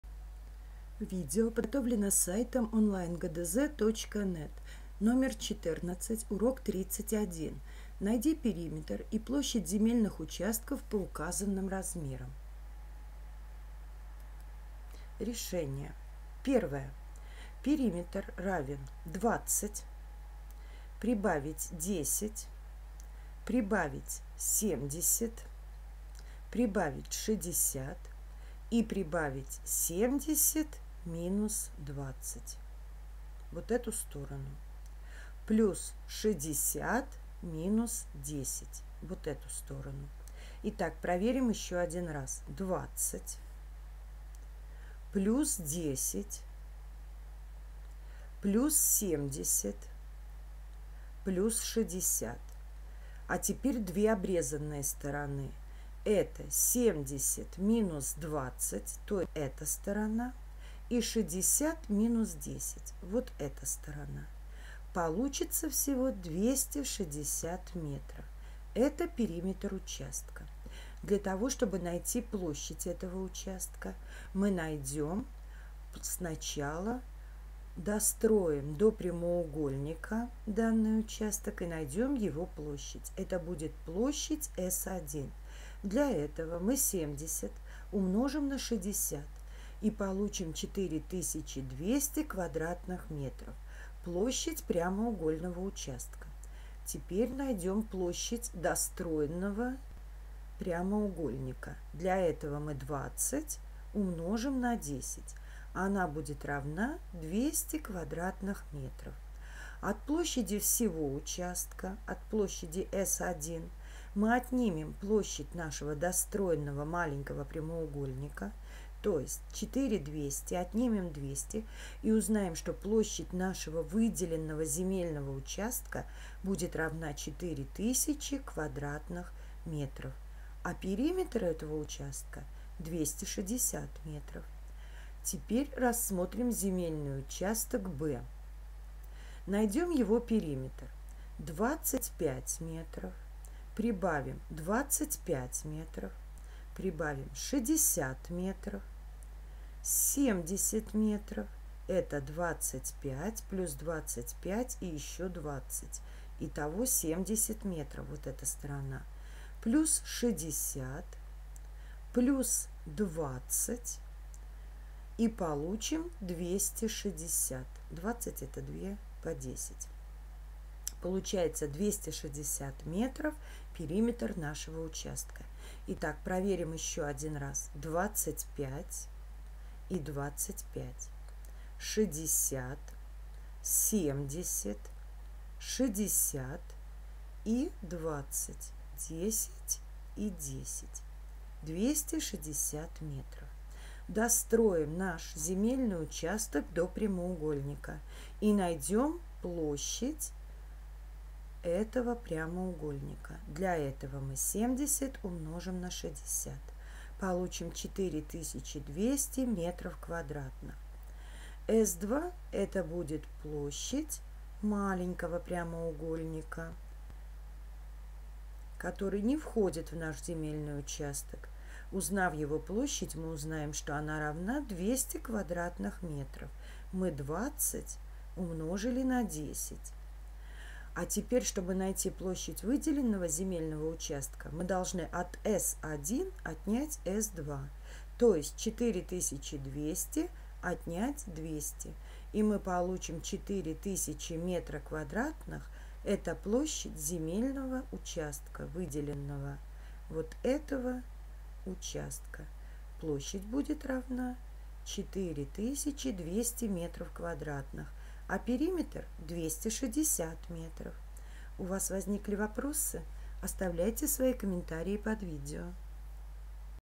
Урок 31 Задание 14 – ГДЗ по математике 4 класс (Петерсон Л.Г.) Часть 1
В данном номере находим периметр и площадь земельных участков по указанным размерам.